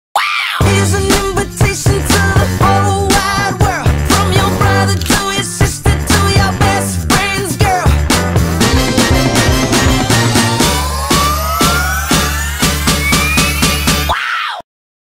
Samsung Galaxy S9/S9 Plus- The Camera. Reimagined.
Presenting a camera that adapts like the human eye. The Dual Aperture lets you shoot stunning pictures, from broad daylight (F2.4 aperture) to super low light (F1.5 aperture).